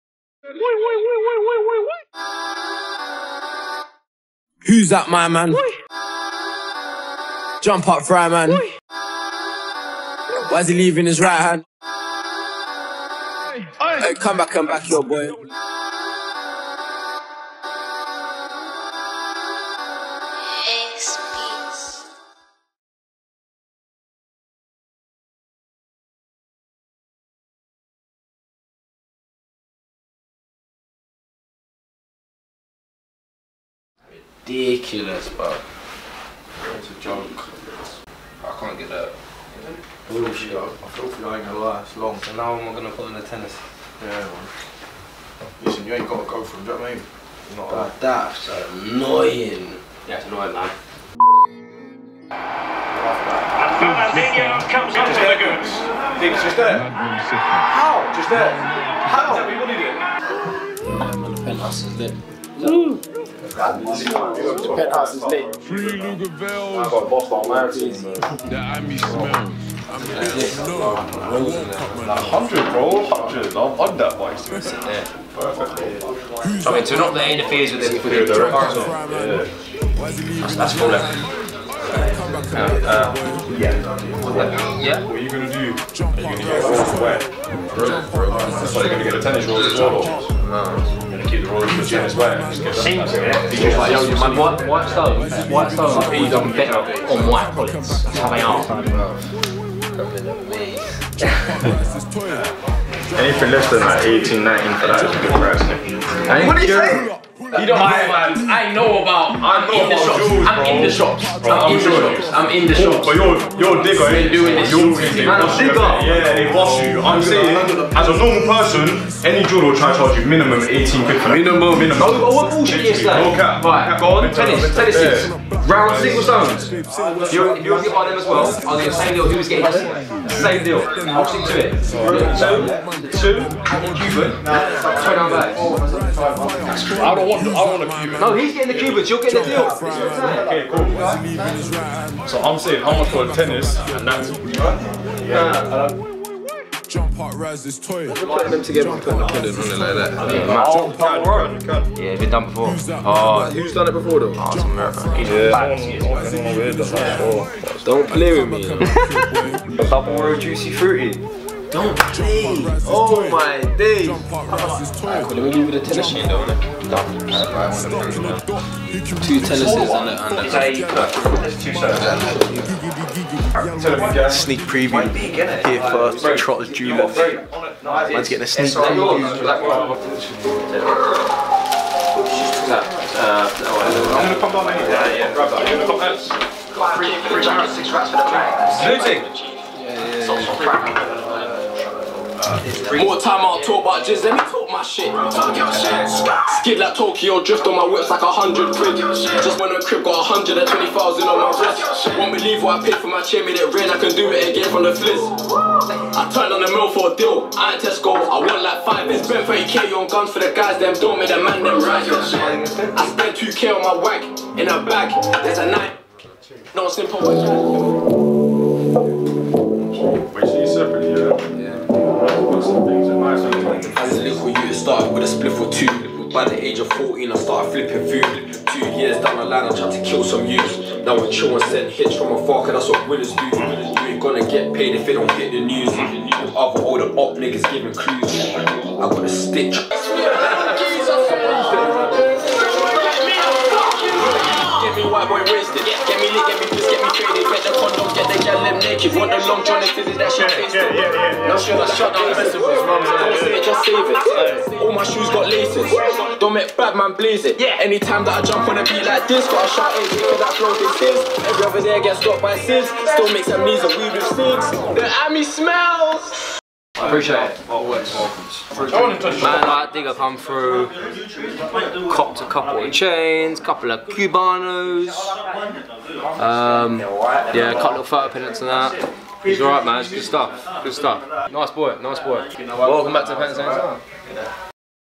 Digga D goes jewellery shopping | Digga D Teases New Music | Trotters Jewellers
Yo guys

A bit of a special video today, our boy Digga D popped down to the Penthouse to check us and get some pendants. Unfortunately, whilst in the Penthouse, Digga was told by police the pendants he wanted would be a breach of his licences. Despite this, we still had a good time with Digga and we had plenty of things to interest him.

We also got a sneak peek at some of Digga's new hits and had a good time with the lads. Digga D, thanks for coming through, welcome any time.